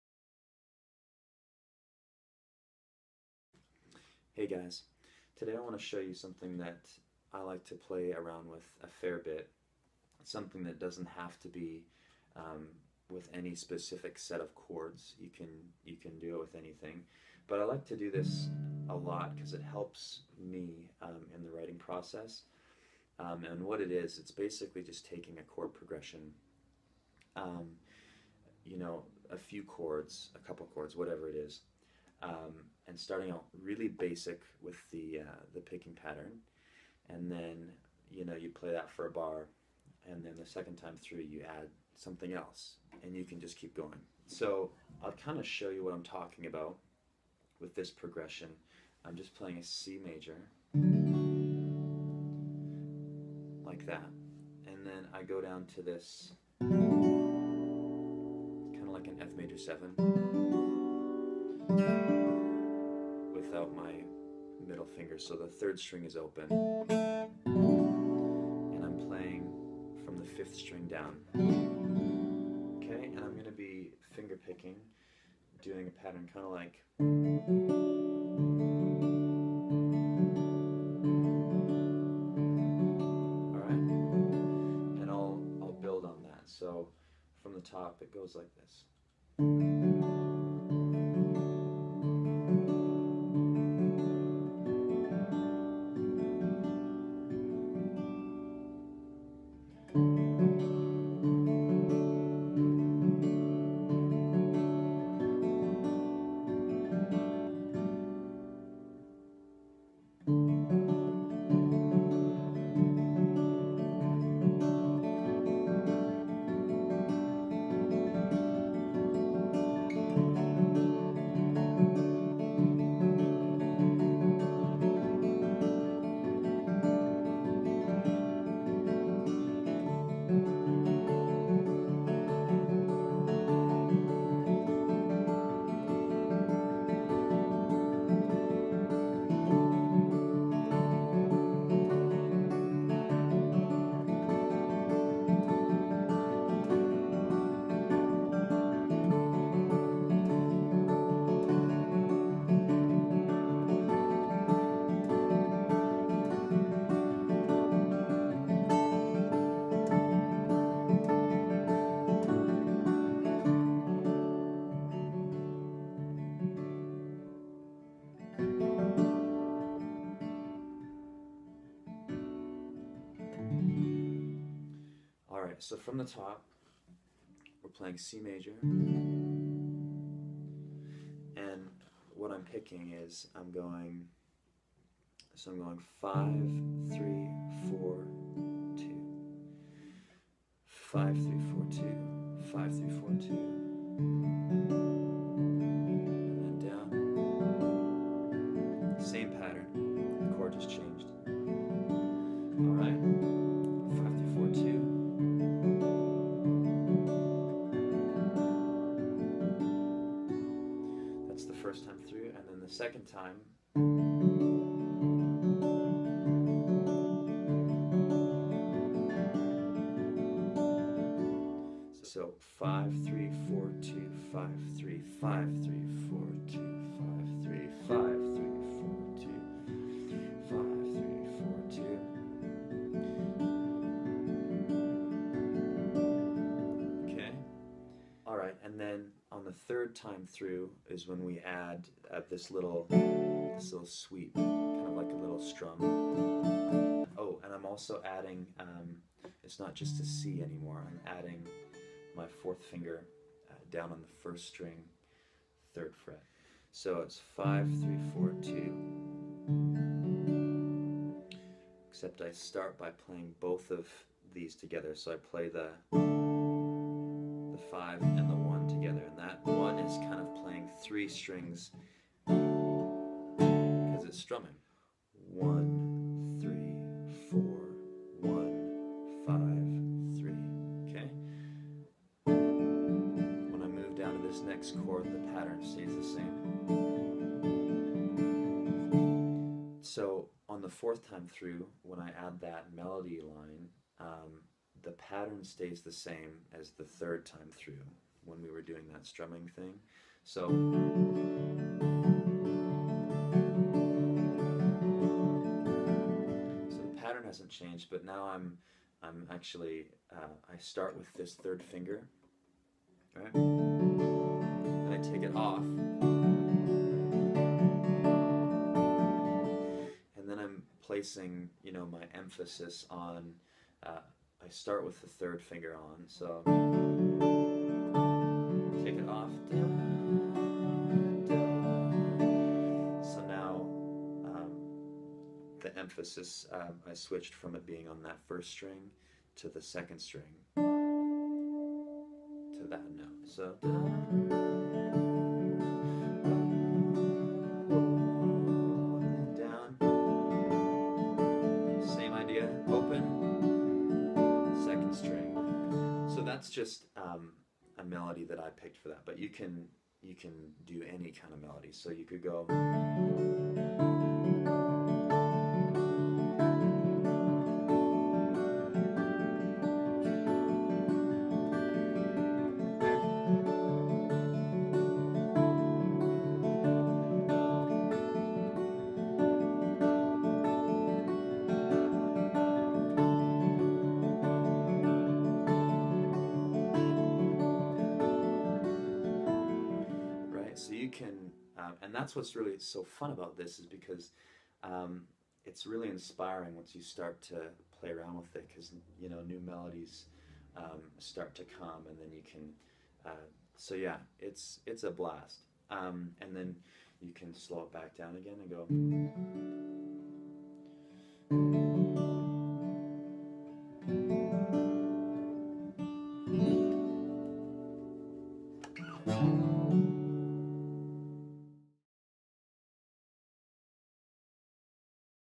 Be creative with an acoustic guitar.
Tips on strumming and how to be creative with an acoustic guitar.